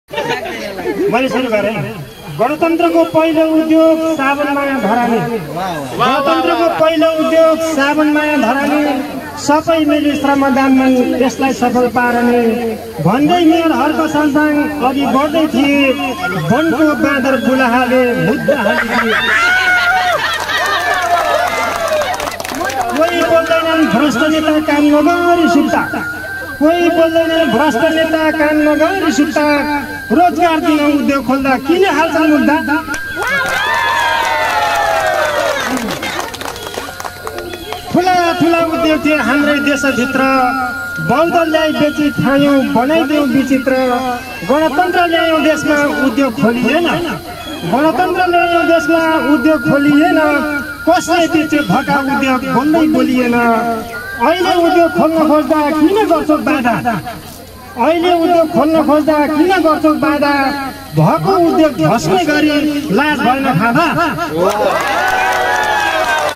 ठीक समय मै आयो साह्रै कडाआयो भ्रष्टाचारहरु बेहोस भए होलान सुनेरो ।👏👏🙂
.ठीक समय मै आयो साह्रै कडा कबिता भ्रष्टाचारहरु बेहोस भए होलान सुनेरो ।नमस्कार यहाँहरु सबैलाई 🙏 यदि तपाई नयाँ हुनहुन्छ भने कृपया:अरु नयाँ नयाँ भिडियो आएको जानकारी पाउनको लागि भिडियोलाई Like गरेर् यो च्यानललाई SUBSCRIBED पनि गर्नु नबिर्सनु होला।धन्यवाद।